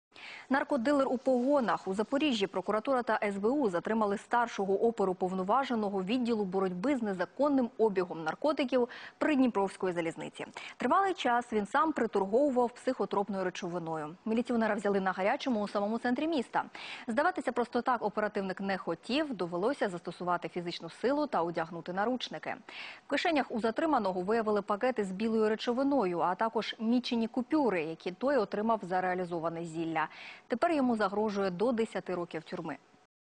Запорожская СБУ задержала милиционера-наркодилера
Запорожская СБУ задержала милиционера-наркодилера - Новости. Утро  - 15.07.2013

Zaporizhzhya SBU detained by policemen-husband - news. Morning - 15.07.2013